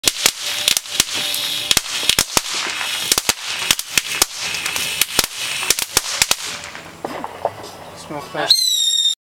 fireworks21
w/maria & casey & mom @ rockford ones, & at aunt cheryls house for illegal fireworks!